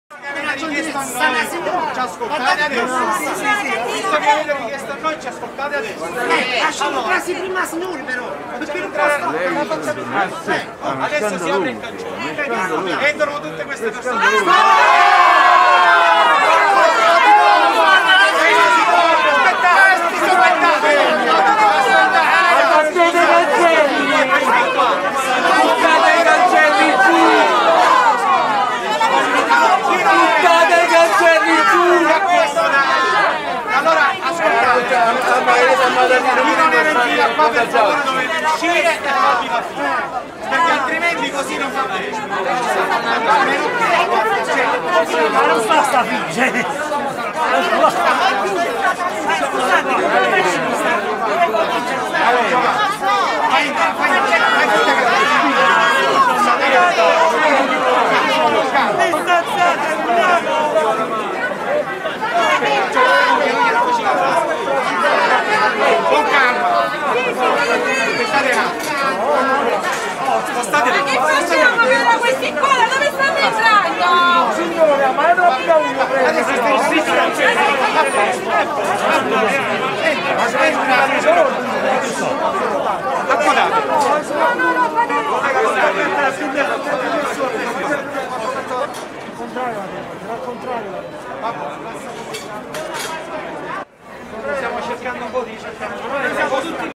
Vaccini, ancora caos alla Mostra d'Oltremare a Napoli
Persone in fila ammassate come sardine

 (Napoi). Ancora caos e tensioni alla Mostra d'Oltremare di Napoli. Centinaia di persone in fila, ammassate come sardine, hanno perso la calma quando per contingentare gli ingressi sono stati chiusi i cancelli. È stato necessario l'intervento delle forze dell'ordine (Luca Leva/alanews)